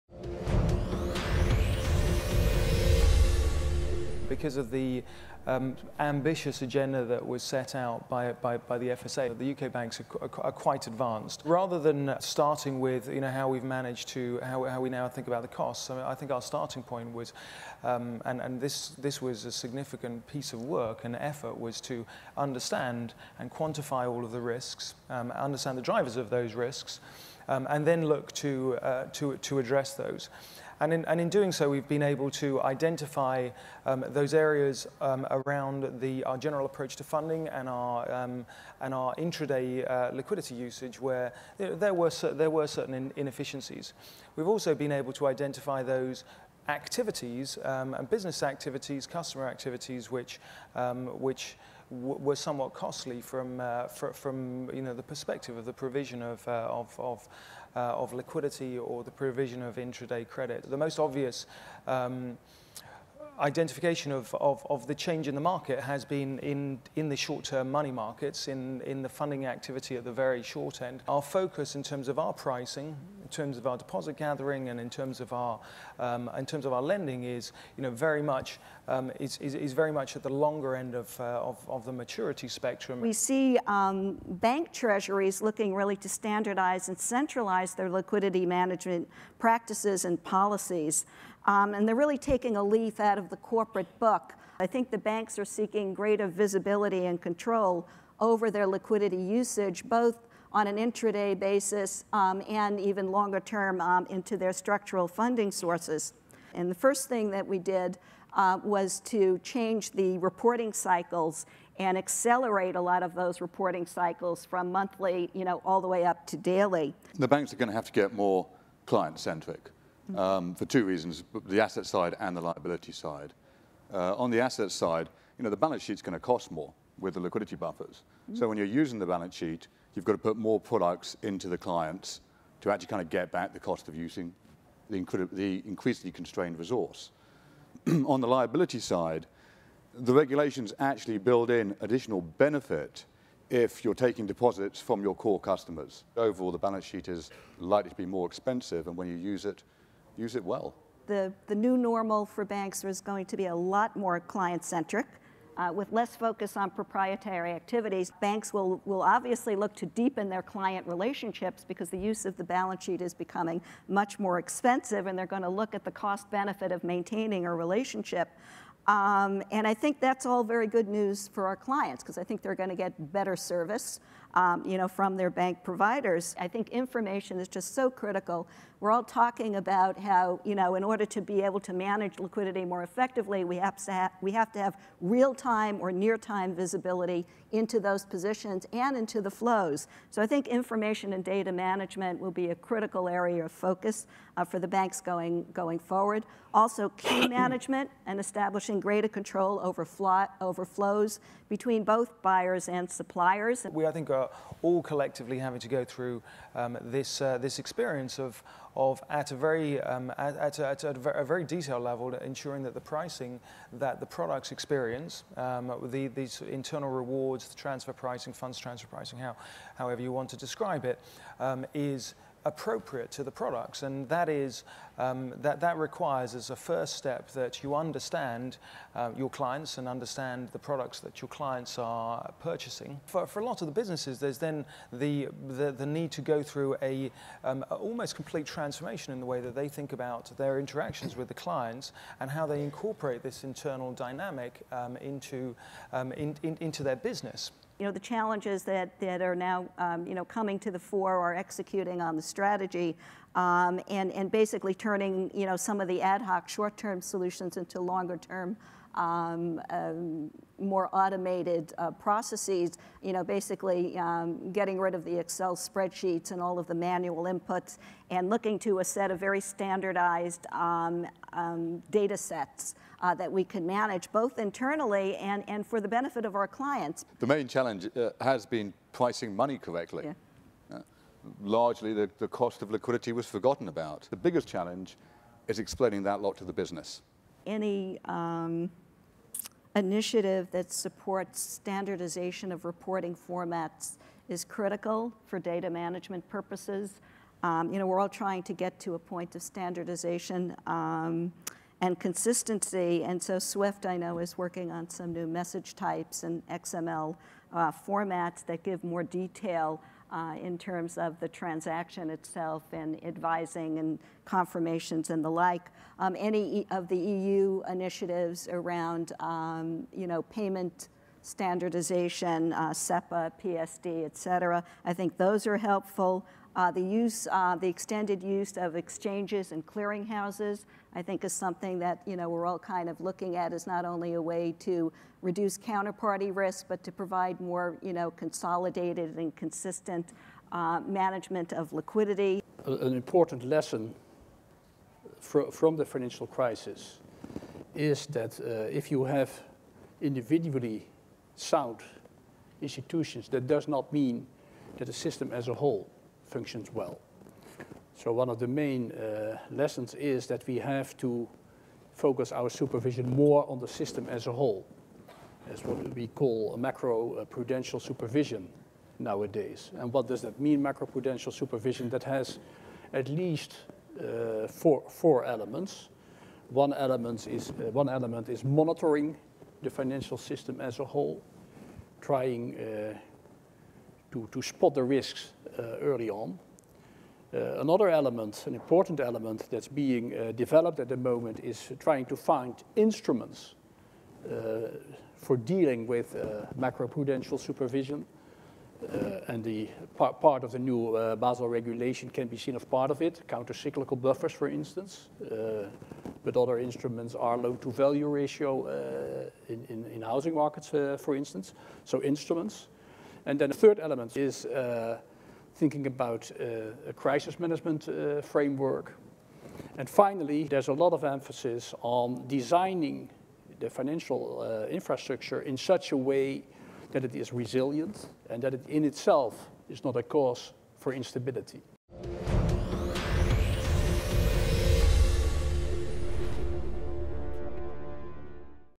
Liquidity risk management
Overview on the debate on the challenges at hand in managing liquidity risk